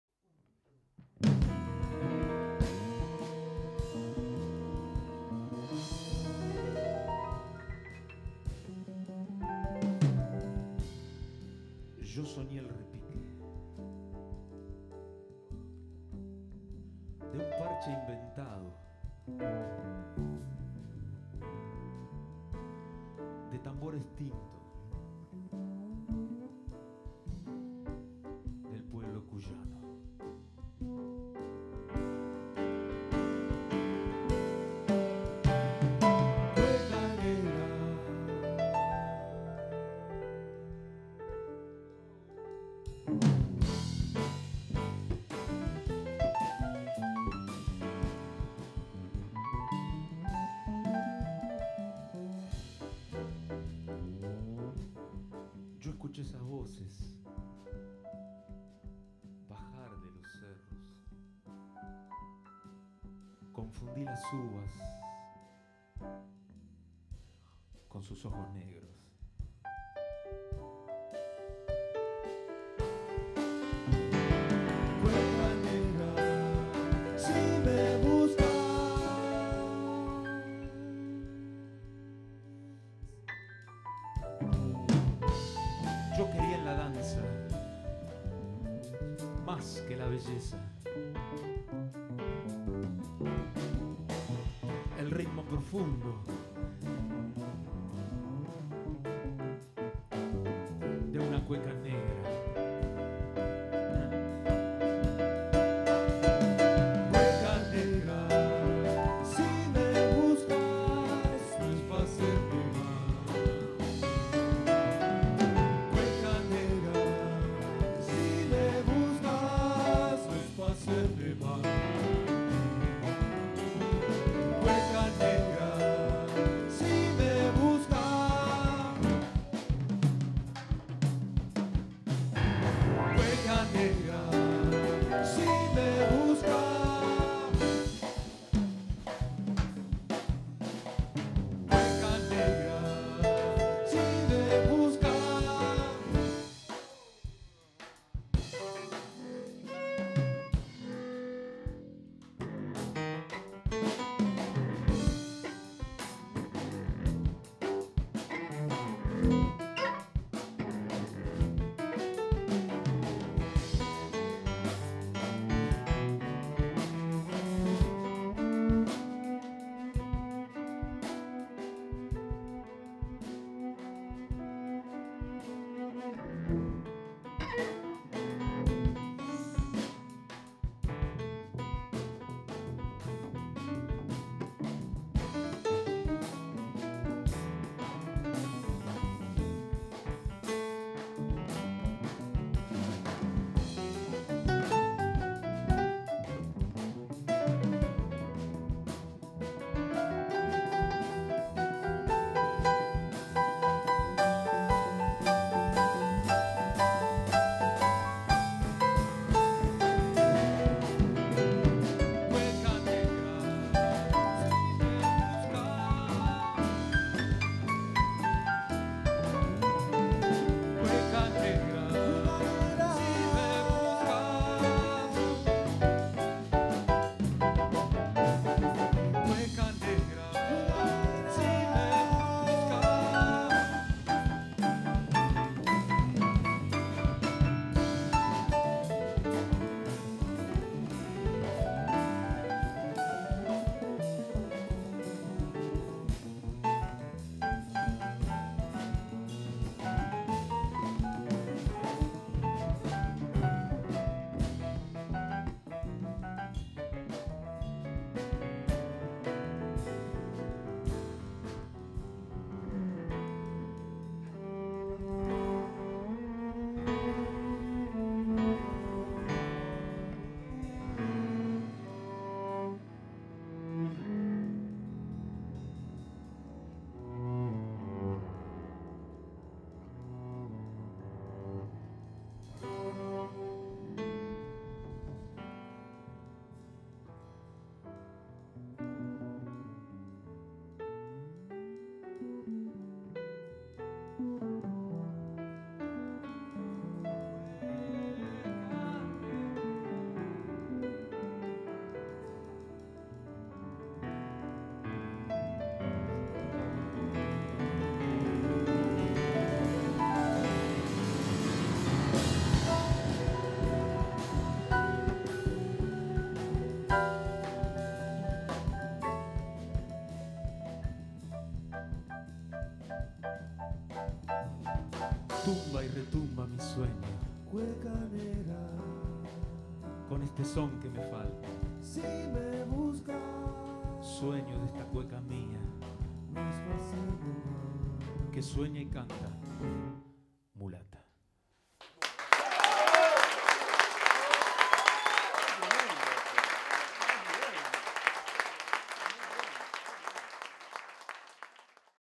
PABLO TOZZI TRIO
Un clip para recordar una presentación que nos emocionó: Pablo Tozzi Trio, con Sebastian Jakimczuk en piano y Néstor Lamónica en batería junto a Maria Antonia Policicchio como invitada especial. Para disfrutar.